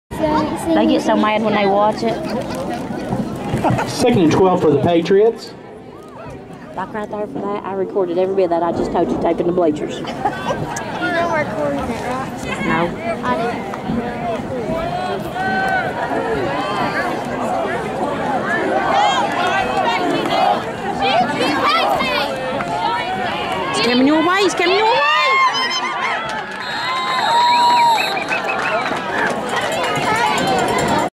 Blake Spencer #21 soph year Betsy Layne Football
#21 Blake Spencer Defense and Offense vs. Knott Central